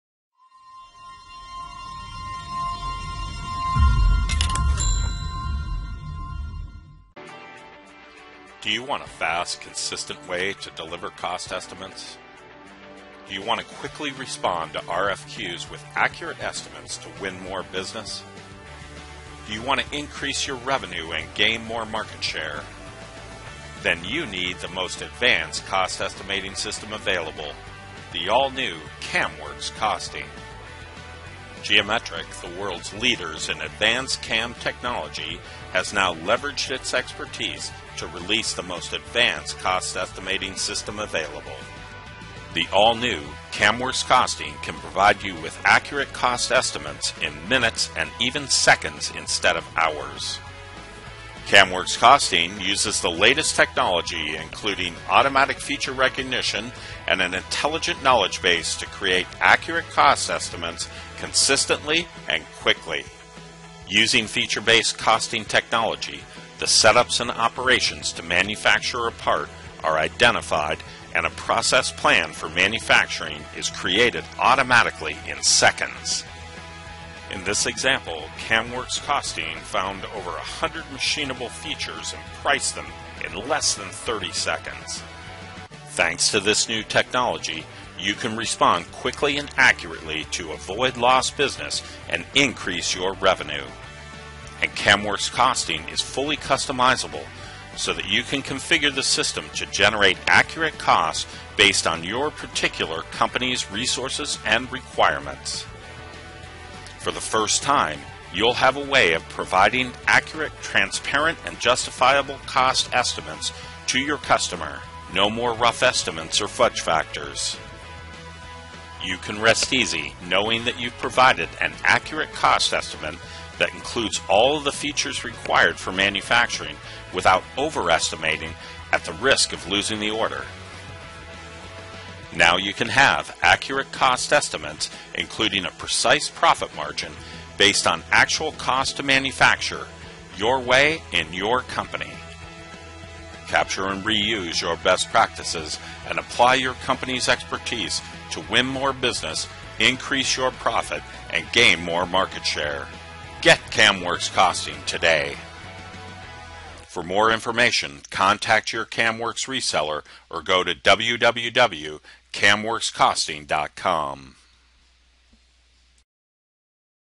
CAMWorks Costing - Manufacturing Cost Estimating Software
Manufacturing Cost Estimating Software

Feature-based driven cost estimation software. Combining the power of Excel with the intelligence/awareness of the solid model!